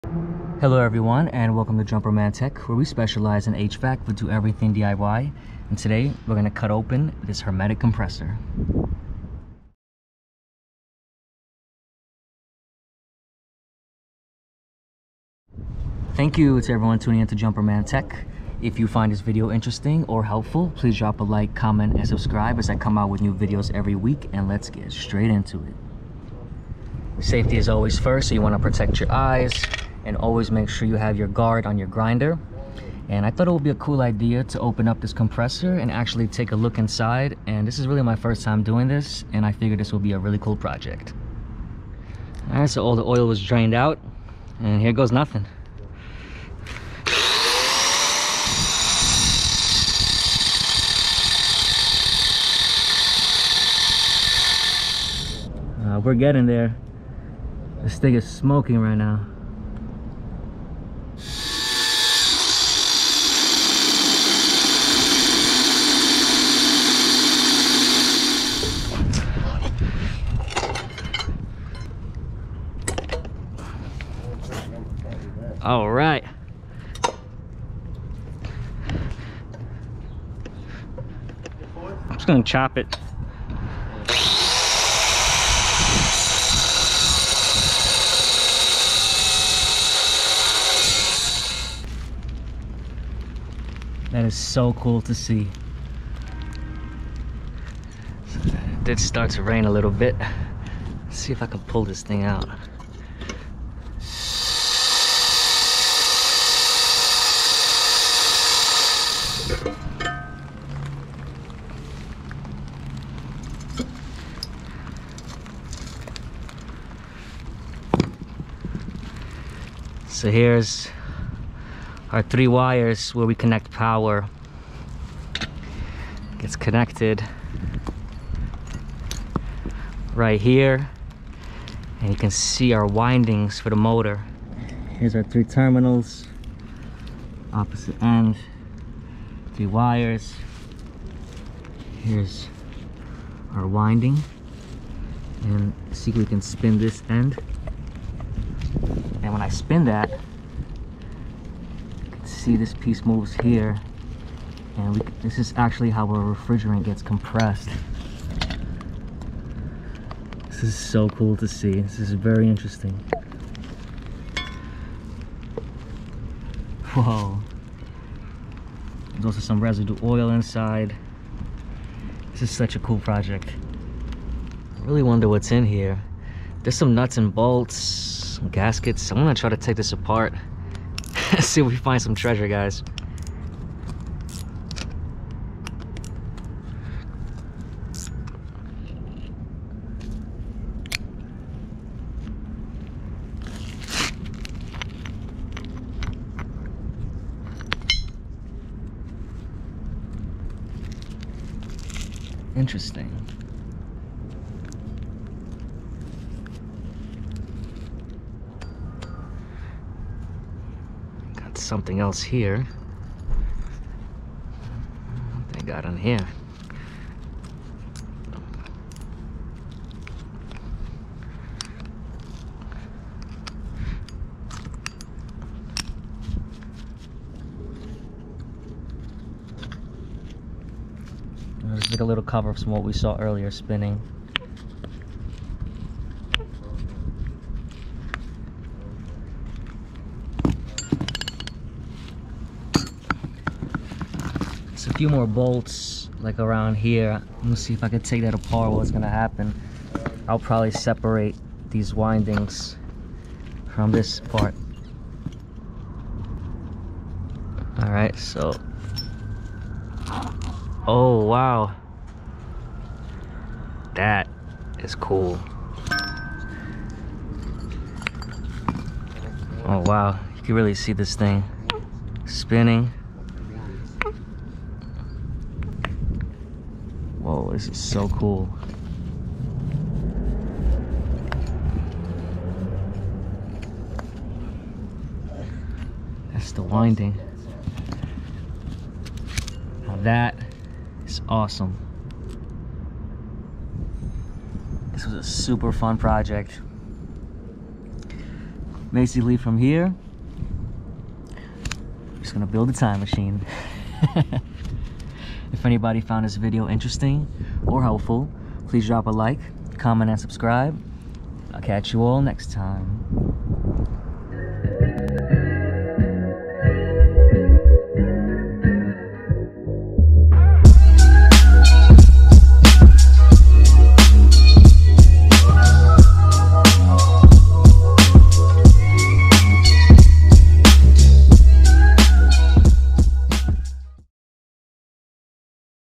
HVACR: Cutting open HVACR Hermetic Compressor (Air Conditioning/Refrigeration Compressor) MUST WATCH
Today we are going to cut open a AC/Refrigeration hermetic compressor!